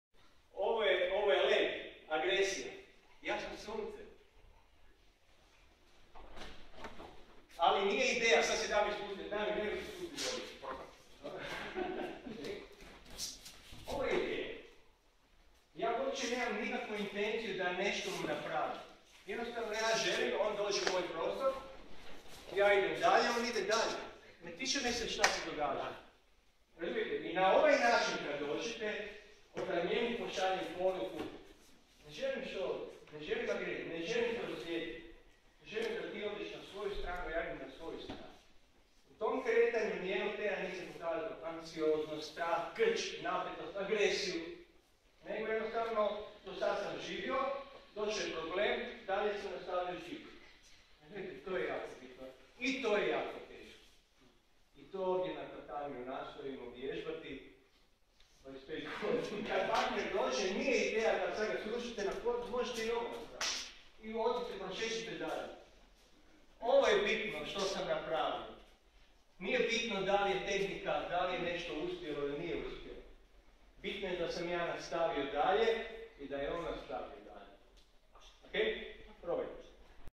Prva aikido lekcija: Neborbeni um / Kako se nositi s problemima
U svijetu punom nasilja i nasilnih odgovora kao pokušaja da se to isto nasilje zaustavi, aikido pruža odgovore i rješenja nakon kojih obje sukobljene strane imaju priliku nastaviti sa životom na najbolji mogući način. Njegovati neborbeni um ne znači bježati od problema, upravo suprotno - s neborbenim pristupom se suočavamo s problemom, ali na način da ulažemo minimalnu količinu energije za maksimalan rezultat. Ne trošimo puno, a dobivamo i više nego smo očekivali :) Snimka je s uvodnog treninga novih članova u ožujku 2017., sensei Marijan Kudrna.